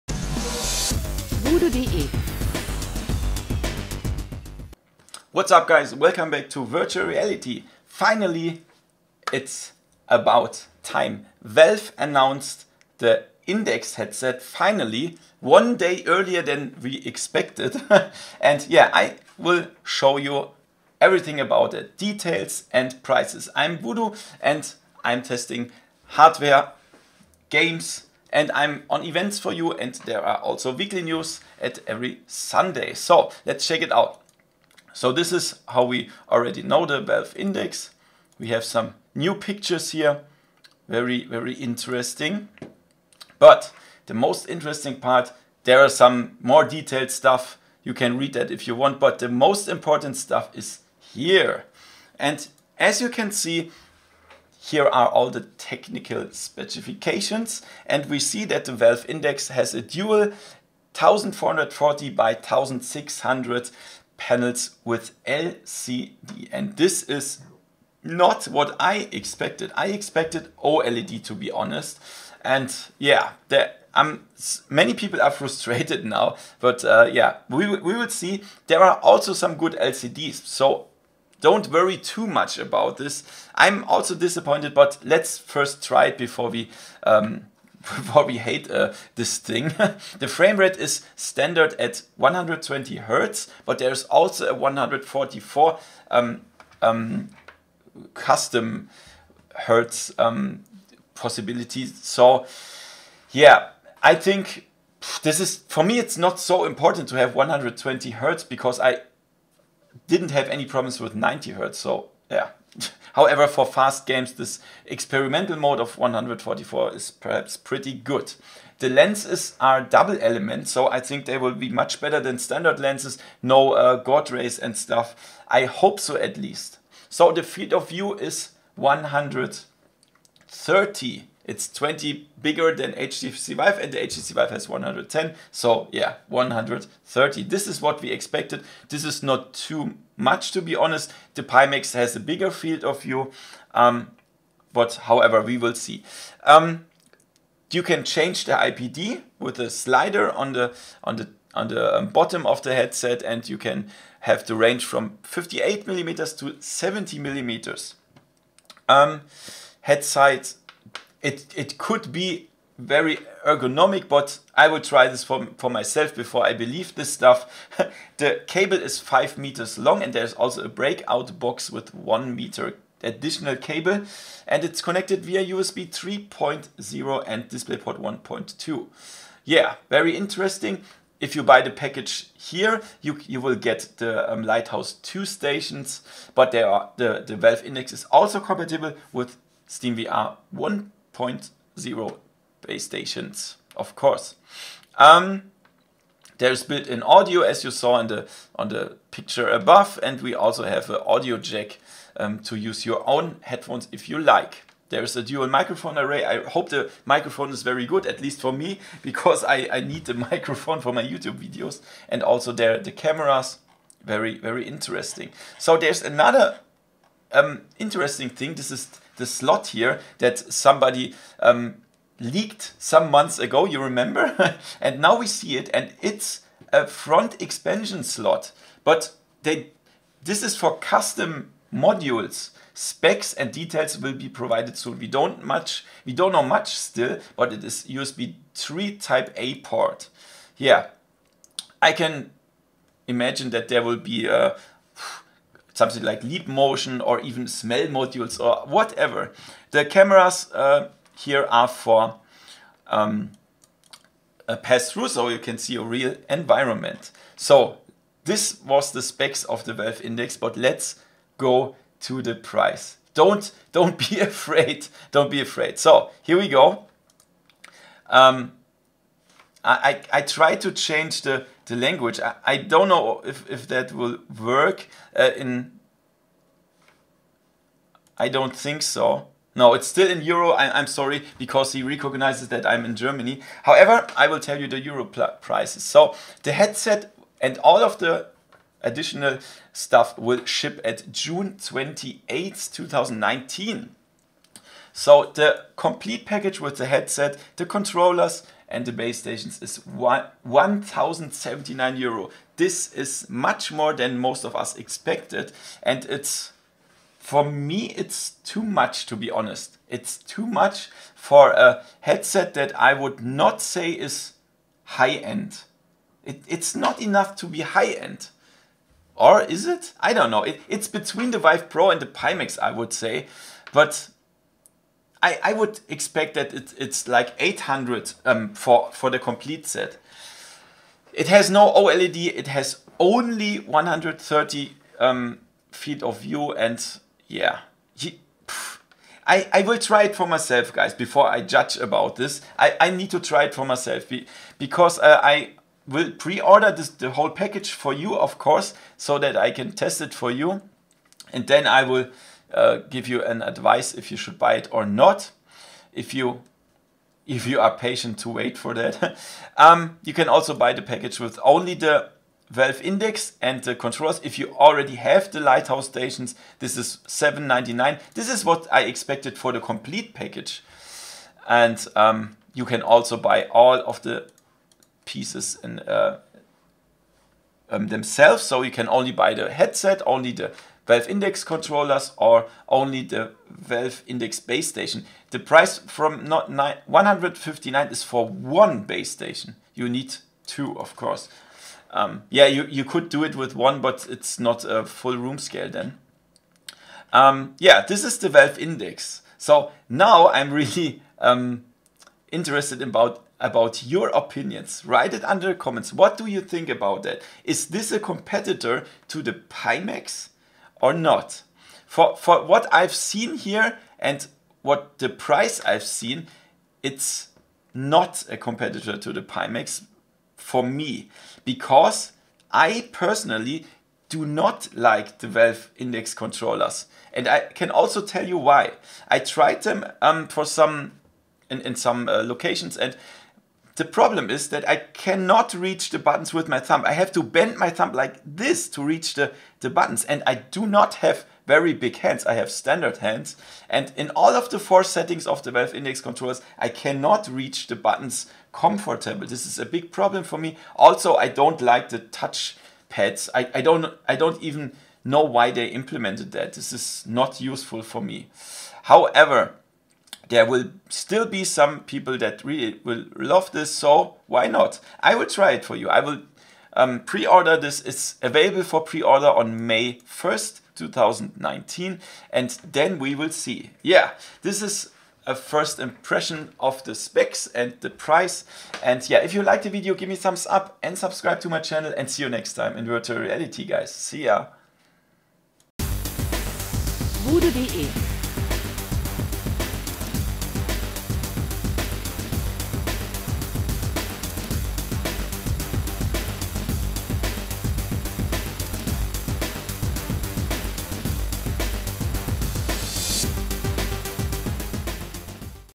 VALVE HAS ANNOUNCED THE INDEX VR HEADSET- All specs, prices and release date!!
Valve has finally announced the details on the Valve Index VR glasses, as well as the controllers and base stations.

vrforum.de
mixed.de

► My rig
Here you can find the recommended Hardware by VoodooDE in my personal Amazon Shop:

(Source for Thumbnail-Picture und description text: Steam Shop)